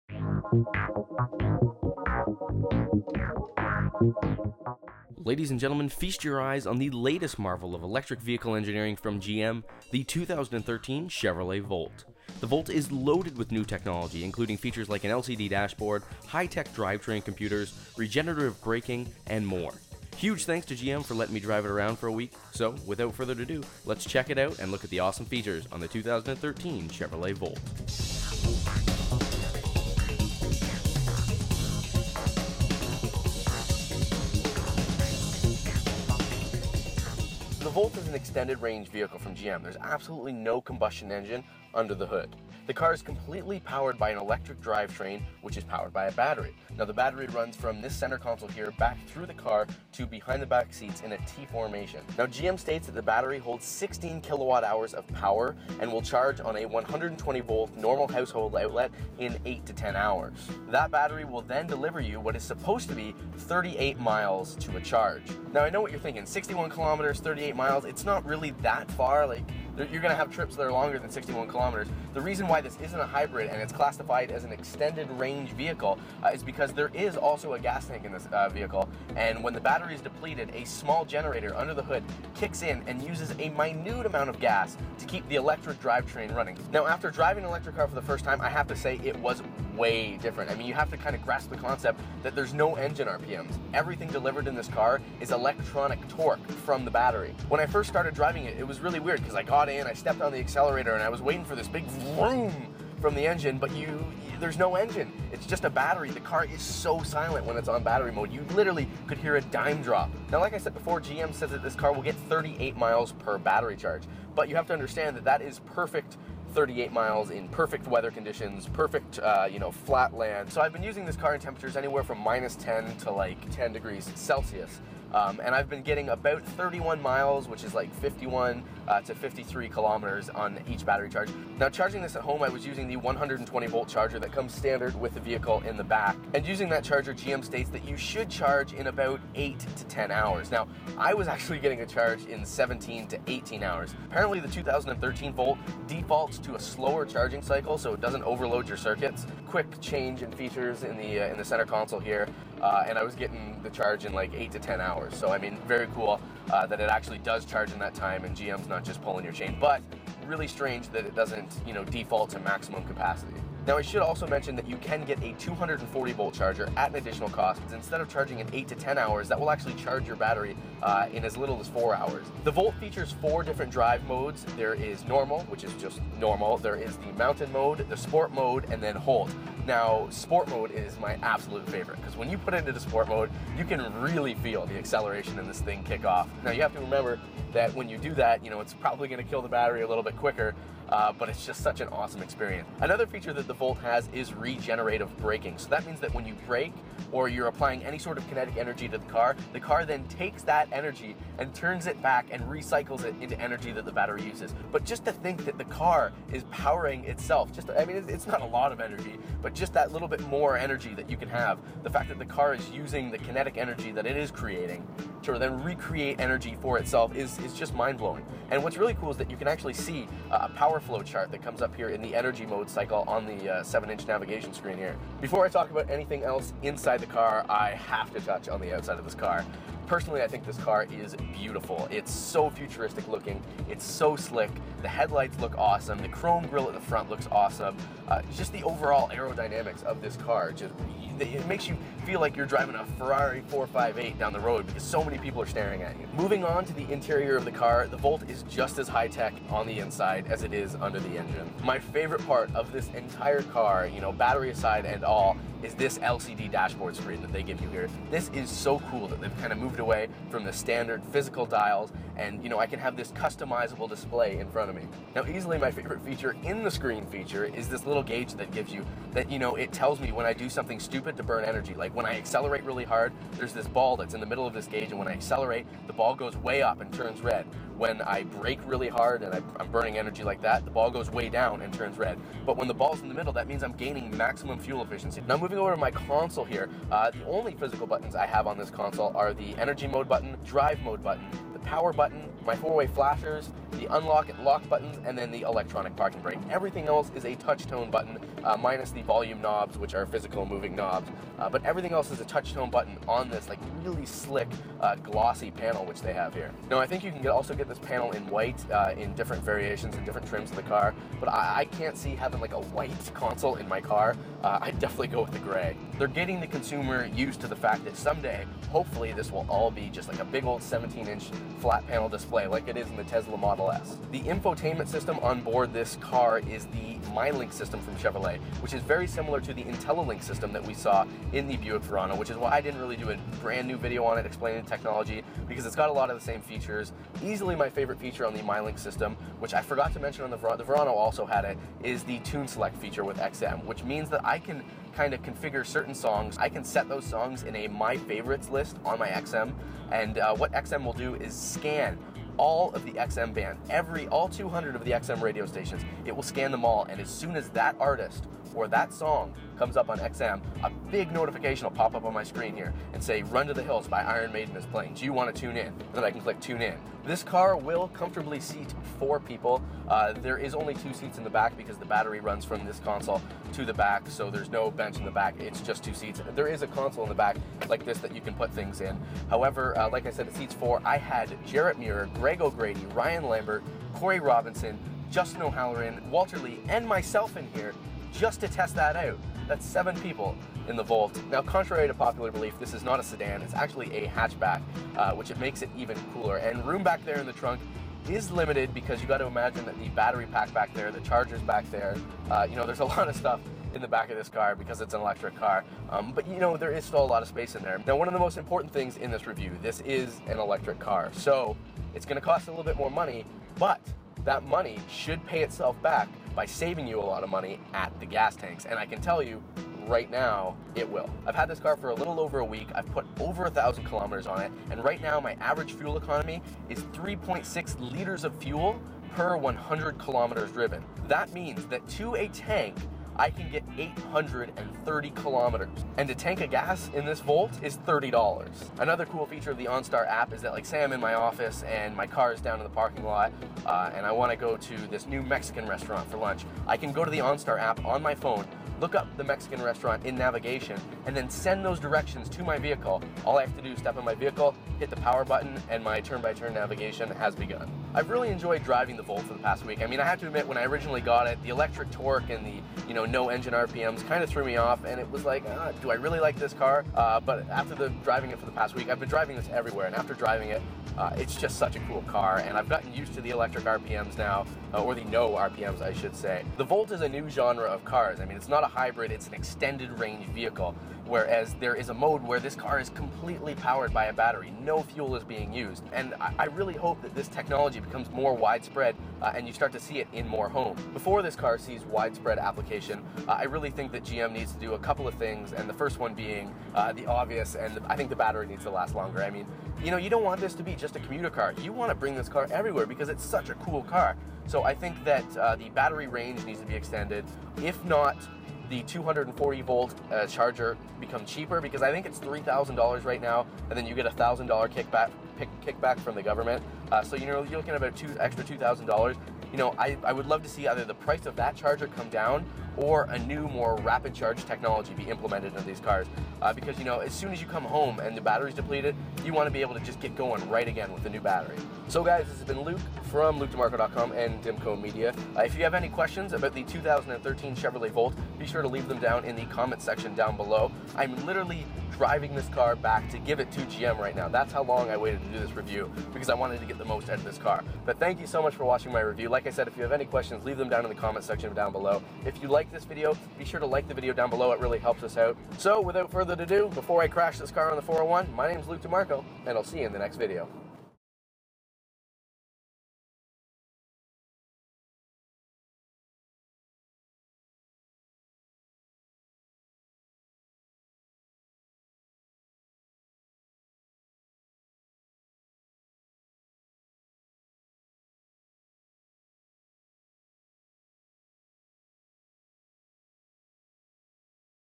AutoTech - 2013 Chevrolet Volt Review
- Feast your eyes on the latest marvel of GM EV engineering, the 2013 Chevrolet Volt. The Volt is loaded wit new tech and features including LCD dashboards, on-board computers, and kinetic energy renewal. Huge thanks to GM for letting me drive it around for a week, looking forward to picking up the Chevrolet Spark for review this week.